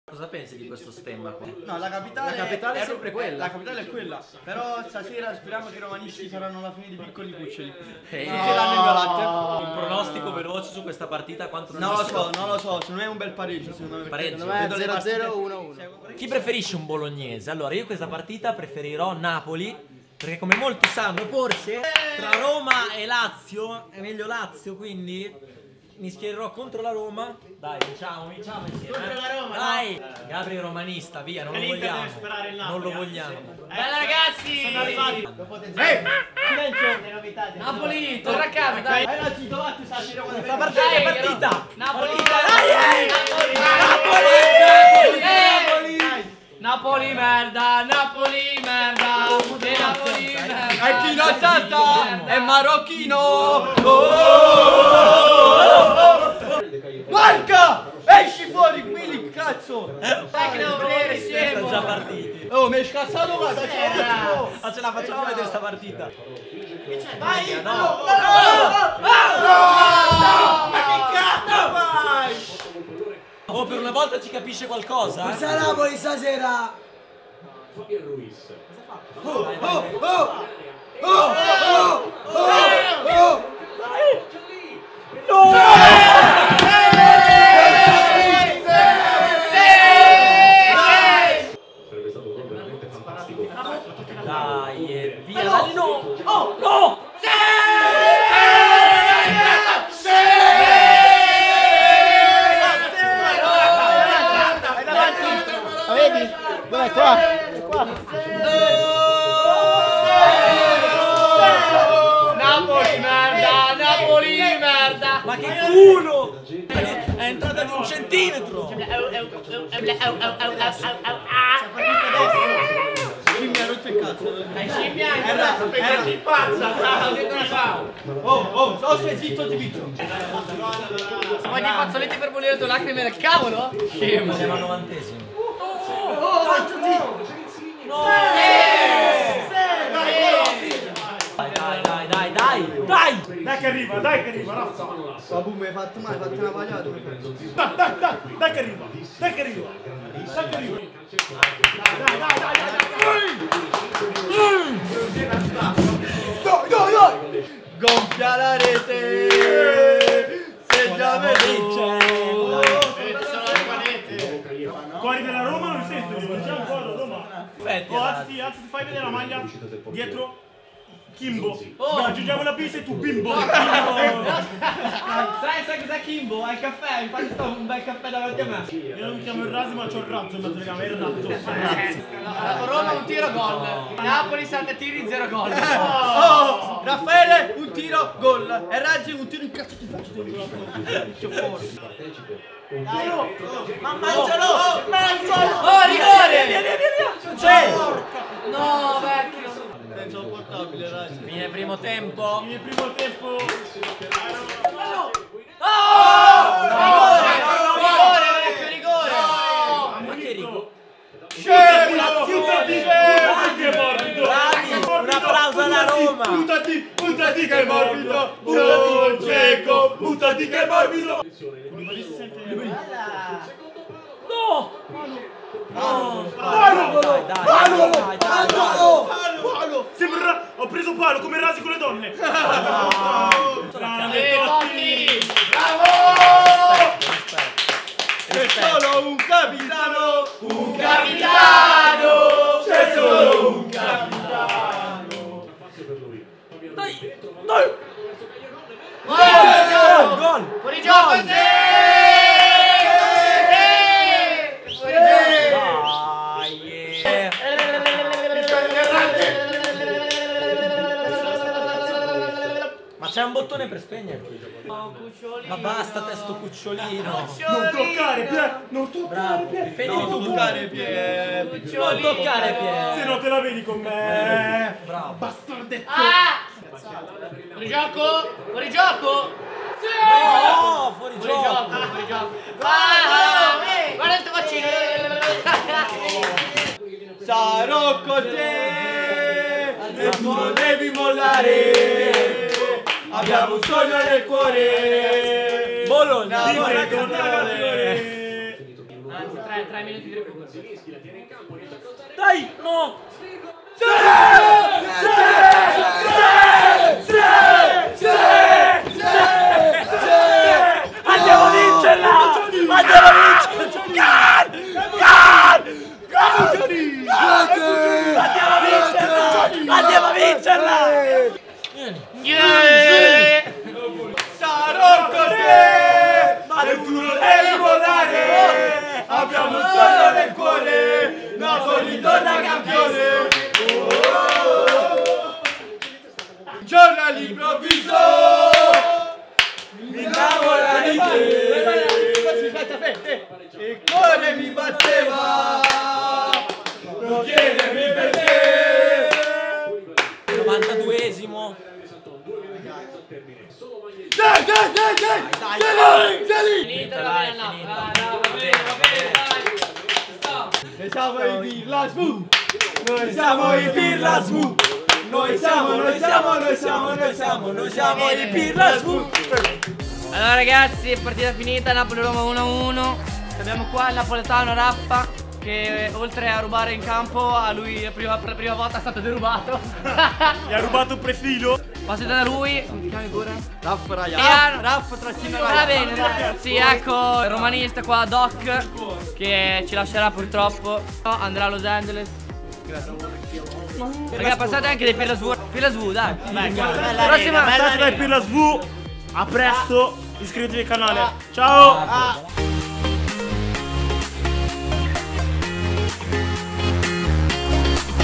NAPOLETANO DELUSO! Napoli 1-1 Roma REACTION Della Partita - Napoletano VS Romanista
Napoli Roma 1-1! Ecco qui la nostra reazione alla partita! Il pareggio è stato un risultato giusto per questa partita? Oppure chi meritava la vittoria tra Roma e Napoli? Faccelo sapere con un bel commento!